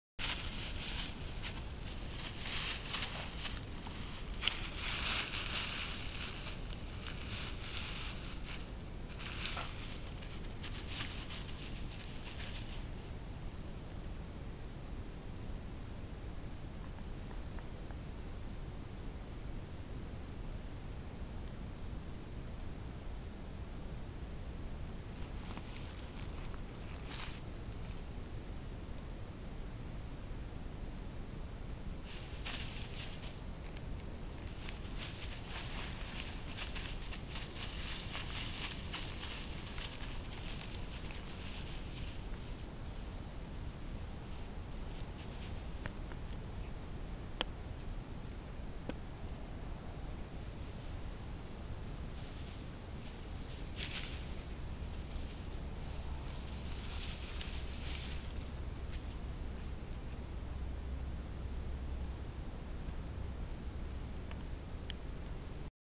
Solea hates her muzzle
As she's been a bit naughty lately on some walks, Solea has to wear her muzzle occasionally. Only trouble is she absolutely hates it and tries to get it off at every opportunity. Today she spent several minutes rubbing her nose in the grass..